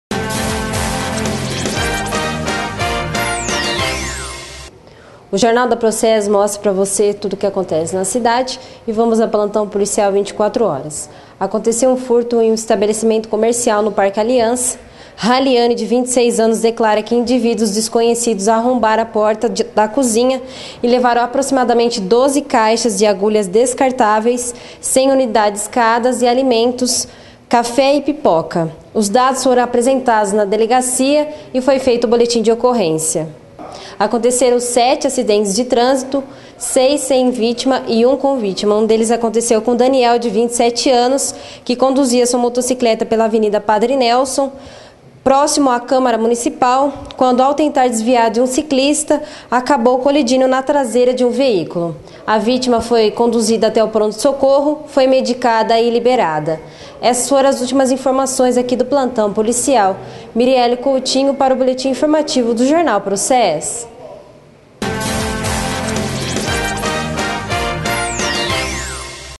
process.com.br - Plantão Policial 18102012
Plantão Policial tranquilo nessa quinta-feira (18)

Os destaques de hoje são: furto em estabelecimento comercial e sete acidentes de trânsito. Confira mais detalhes:
Na manhã de ontem (17) aconteceu um furto em estabelecimento comercial. A ocorrência foi no Programa Saúde da Família do Parque Aliança. Segundo Raliane, 26 anos, declara que indivíduos desconhecidos arrombaram a porta da cozinha e levaram, aproximadamente, 12 caixas de agulhas descartáveis com 100 unidades cada, das marcas Embrac e Solidor, e alimentos da cozinha (café e pipoca). Os dados foram apresentados na Delegacia e foi feito o Boletim de Ocorrência. Até o momento ninguém foi preso.
Em apenas 24 horas, matão registrou mais sete acidentes de trânsito, 6 sem vítima e 1 com vitima. Neste Daniel Fernando Santos, 27 anos, declara que conduzia sua motocicleta pela Av. Padre Nelson quando defronte a Câmara Municipal, ao tentar desviar de um ciclista que cruzava a avenida, veio a colidir na traseira de um veículo. A vítima foi conduzida ao Pronto Socorro, foi medicada e liberada.
Confira os detalhes e as imagens na reportagem de Mirieli Coutinho.
Autora da Matéria:
Mirieli Coutinho